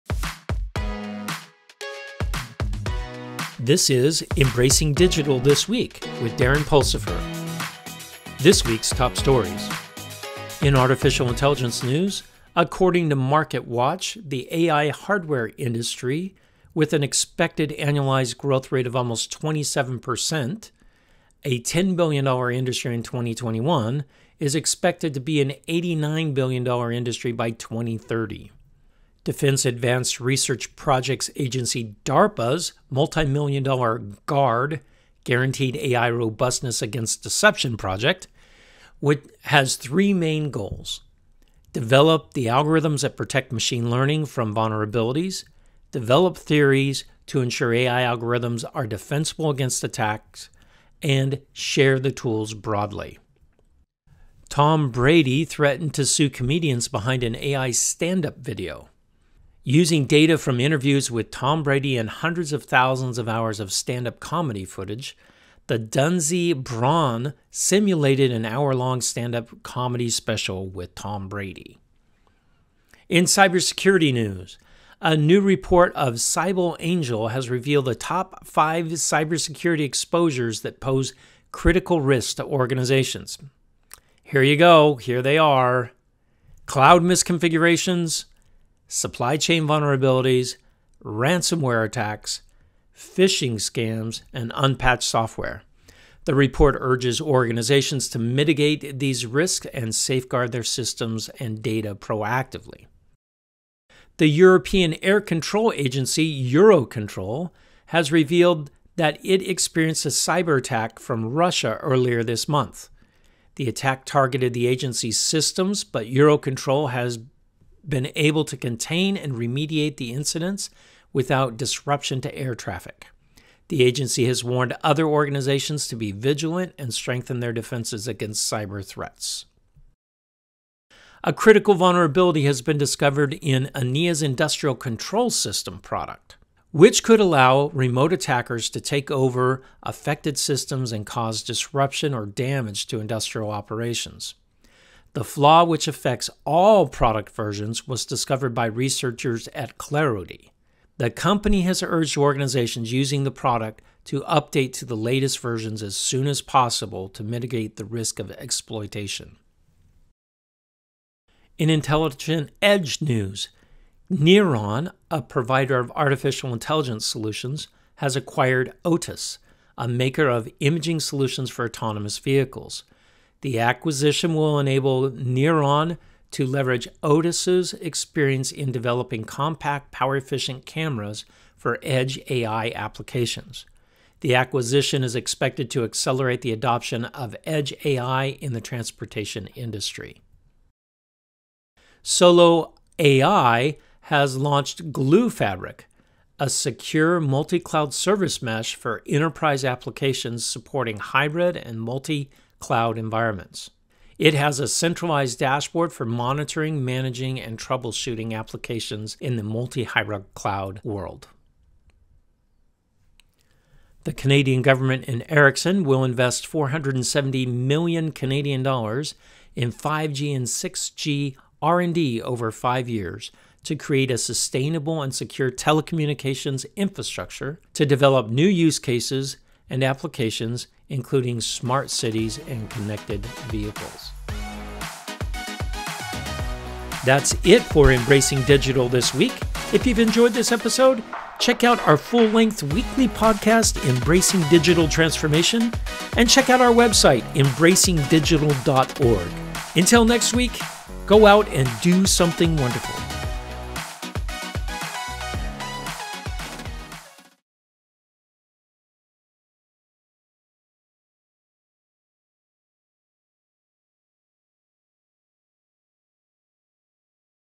Apr 24, 2023 Embracing Digital This Week | Intel Business
Digital Transformation news for the Week of Apr 24, 2023 including Tom Brady's Deep Fake standup comedy, Cyber attacks on Eurocontrol and 470 Million CAD investment in 5G and 6G by Candian Government.